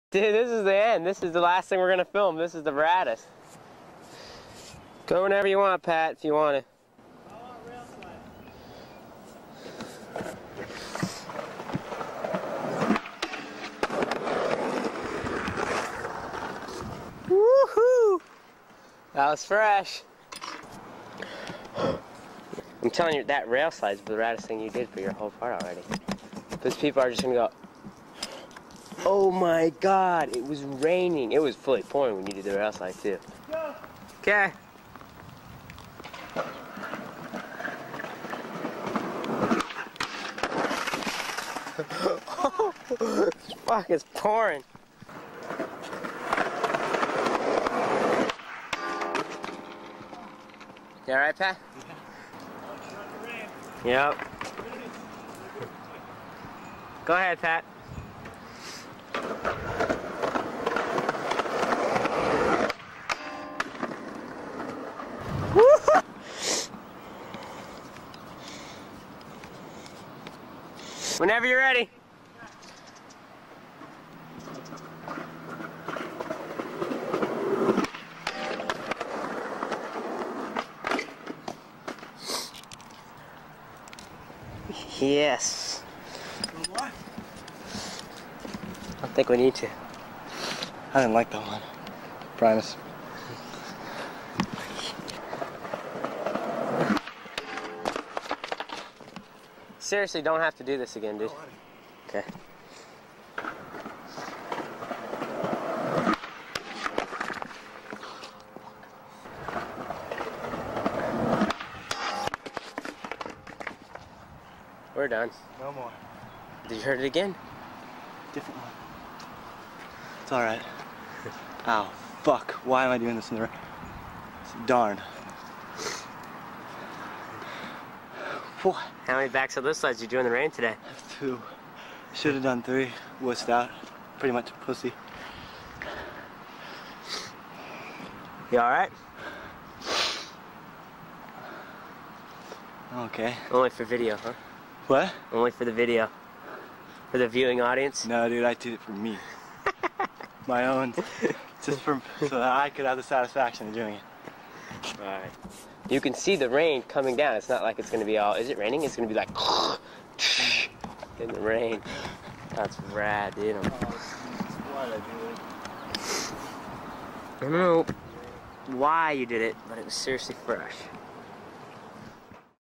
Pat Duffy "I did it for me" - B/S Lipslide in the Rain
"I did it for me, so that I could have the satisfaction of doing it."

My favorite skate clip of all time. The rain, Ternasky hyping from behind the camera, Duffy going back for a second (and third) shot…I love everything about it.

A back lipslide down a 10-stair was unreal for its time. No one was pulling that off 30 years ago, let alone doing it spur-of-the-moment in the rain. It’d still hold up in a video part today.

But what really sets this clip apart is Duffy's answer for why he's skating in the rain.

Full write-up on why I love this clip and what it teaches about internal vs. external motivation